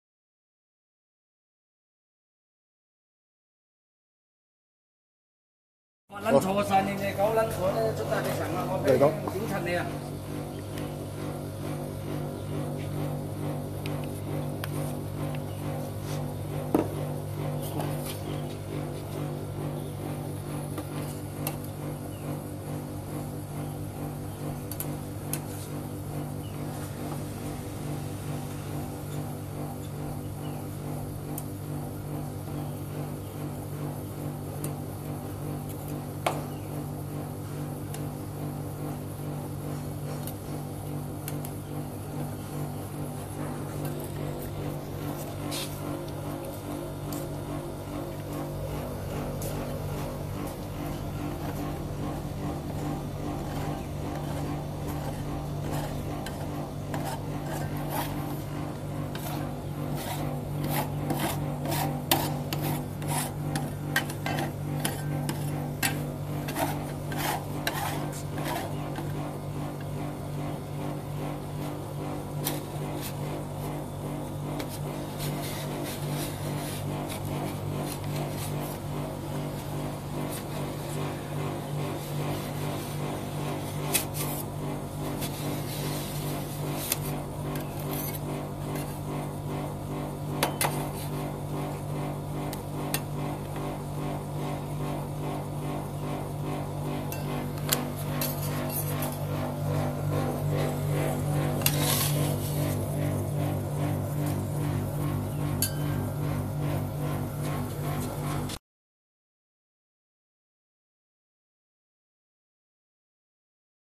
Fried roll ice cream machine from Cremak Co Ltd
Fried roll ice cream machine, with very good quality!